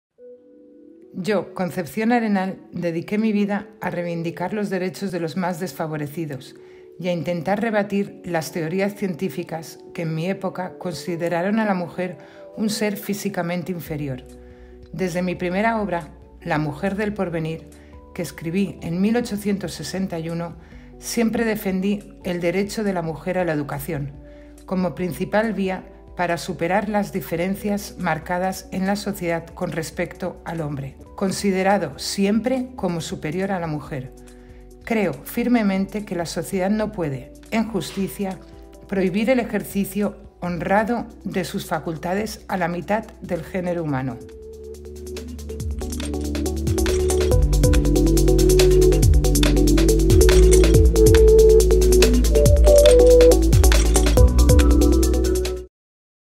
Concepción Arenal, una mujer impulsora de la innovación en los derechos de la mujer
“Las mujeres, impulsoras de la innovación” es un recorrido digital en el que 16 jóvenes de hoy dan voz a 16 mujeres que han tenido mucho que decir en su ámbito de conocimiento, como Concepción Arenal. Una exposición virtual organizada por la Unidad de Igualdad y la Biblioteca-CRAI de la Universidad CEU Cardenal Herrera.